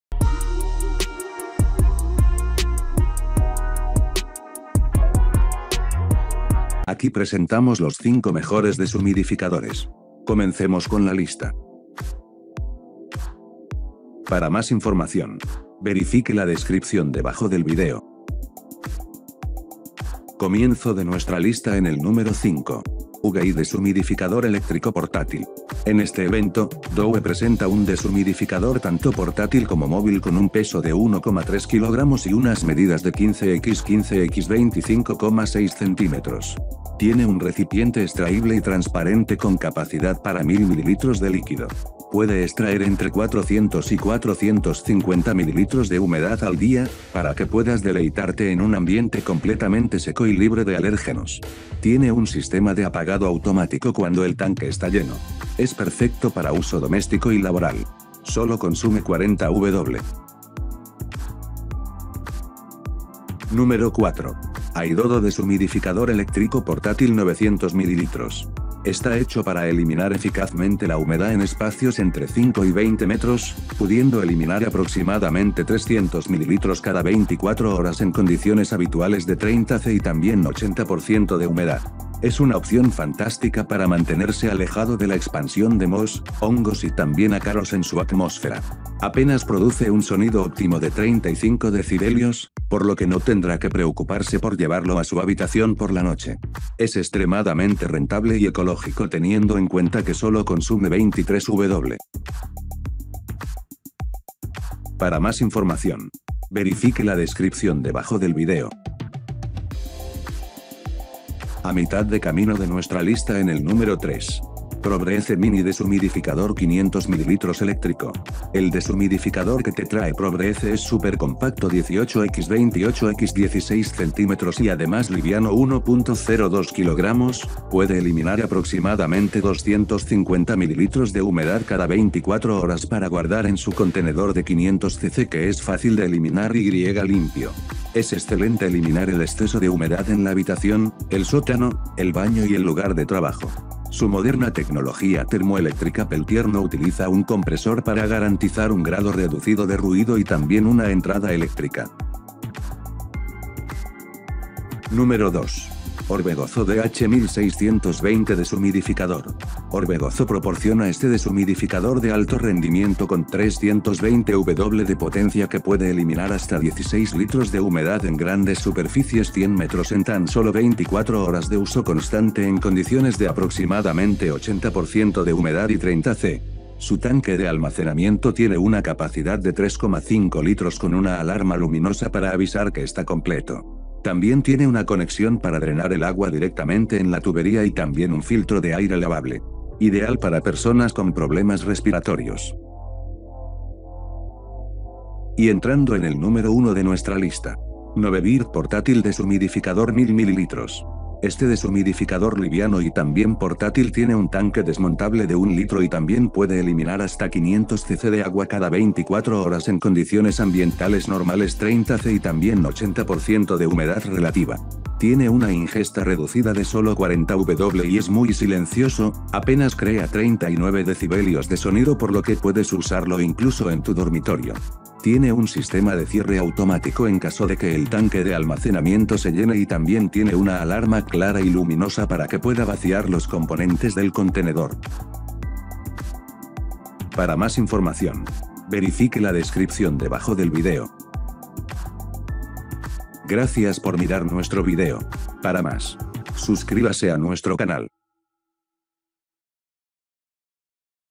Los 5 Mejores Deshumidificadores
Aquí está la lista de Los 5 Mejores Deshumidificadores.

01. Nobebird Portátil Deshumidificador 1000ML

02. Orbegozo Dh 1620 Deshumidificador

03. Pro Breeze Mini Deshumidificador 500ml Eléctrico

04. Aidodo Deshumidificador electrico Portátil 900ml

05. UGHEY Deshumidificador Eléctrico portátil